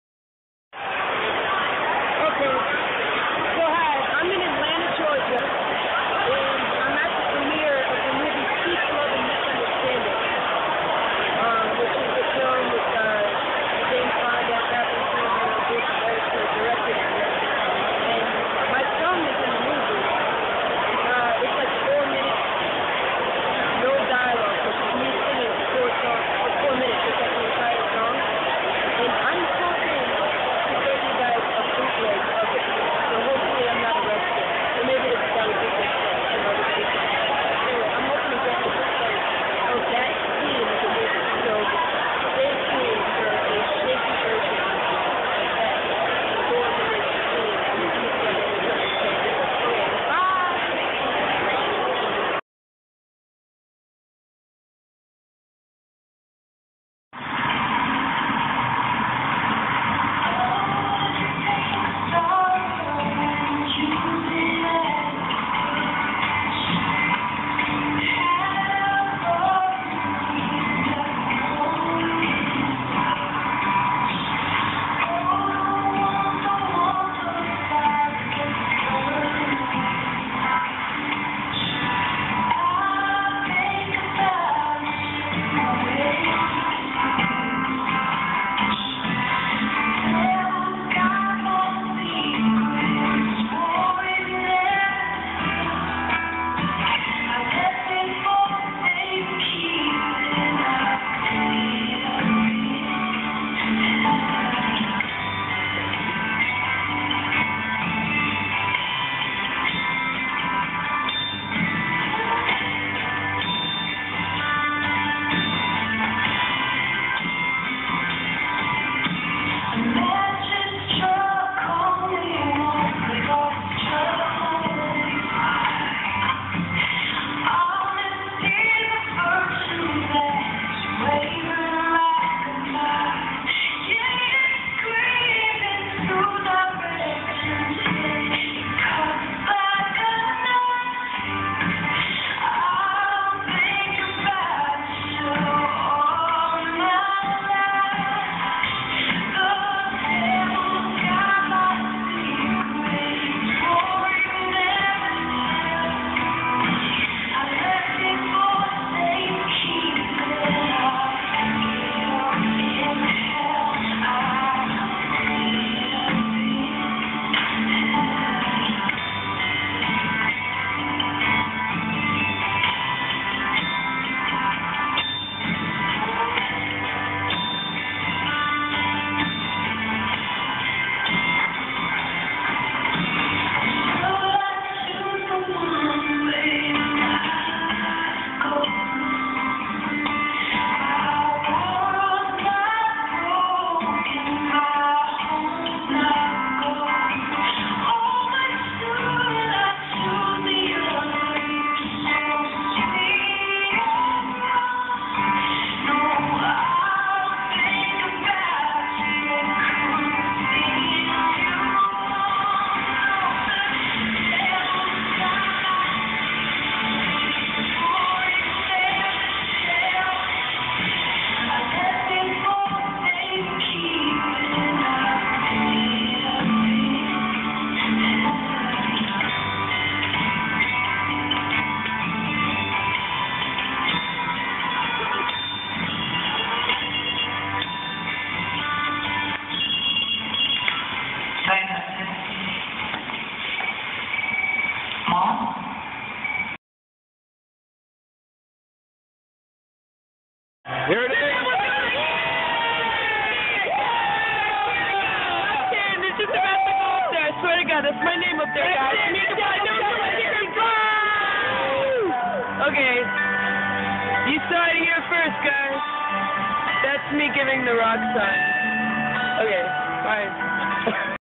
bootleg of my song in a movie
I just got to see the first movie that featured one of my songs ! and here's the bootleg of the scene ! I went to atl for the premiere -- Jane Fonda (the star) introduced the film. got red carpet treatment & everything. enjoy !

I'm going into the studio next week to start recording can't wait for that album to take over the film-soundtrack industry